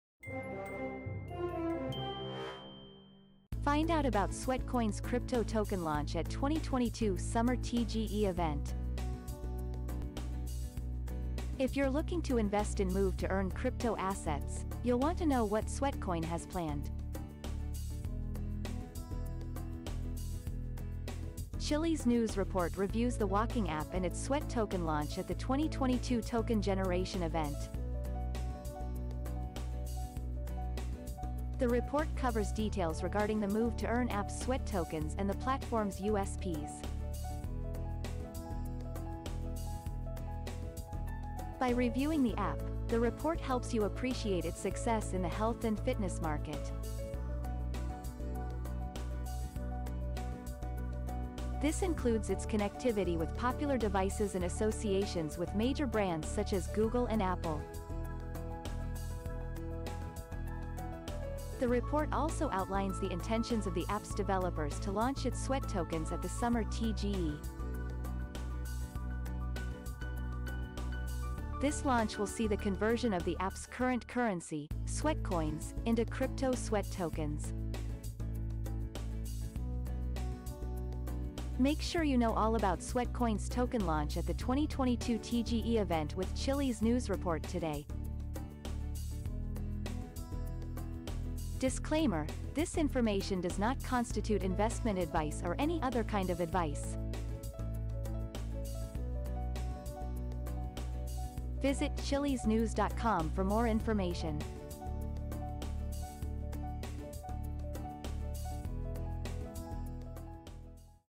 Find Out About Sweatcoin’s Crypto Token Launch At 2022 Summer TGE Event
Do you want to step into the world of Move-To-Earn cryptocurrencies? Would you like to learn more about the upcoming launch of Sweatcoin’s SWEAT tokens? Then read Chiliz News’ report on the latest developments around the project and their much-anticipated Token Generation Event! for more information.